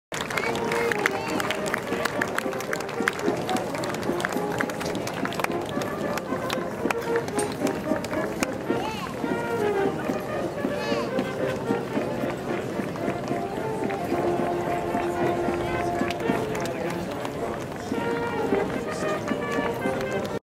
Long shot of crowds marching. Long shot of crowds marchin...
Long shot of crowds marching. Long shot of crowds marching. Australians on April 25, 2013 in Various Cities, Australia